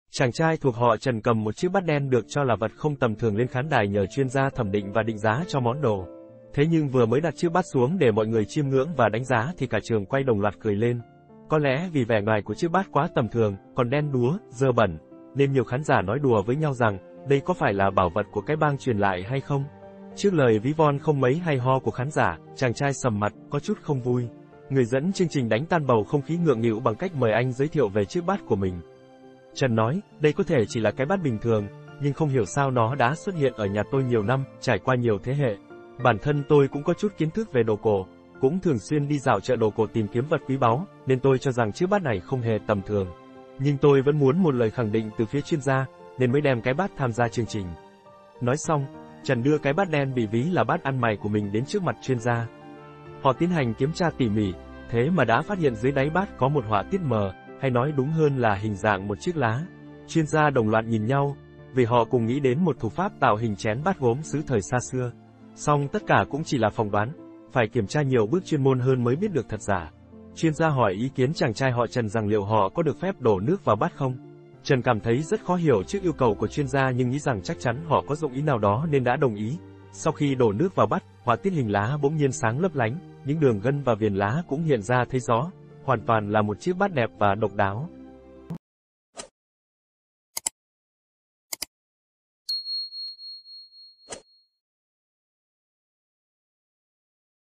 CHIẾC BÁT CỔ BẤT THƯỜNG | TÌM HIỂU VỀ VẬT THẦN BÍ CỦA GIA ĐÌNH TRẦN
Chàng trai thuộc họ Trần cầm một chiếc bát đen được cho là vật không tầm thường lên khán đài nhờ chuyên gia thẩm định và định giá cho món đồ. Thế nhưng vừa mới đặt chiếc bát xuống để mọi người chiêm ngưỡng và đánh giá thì cả trường quay đồng loạt cười lên. Có lẽ vì vẻ ngoài của chiếc bát quá tầm thường, còn đen đúa, dơ bẩn, nên nhiều khán giả nói đùa với nhau rằng: 'Đây có phải là bảo vật của cái bang truyền lại hay không?'. Trước lời ví von không mấy hay ho của khán giả, chàng trai sầm mặt, có chút không vui. Người dẫn chương trình đánh tan bầu không khí ngượng nghịu bằng cách mời anh giới thiệu về chiếc bát của mình.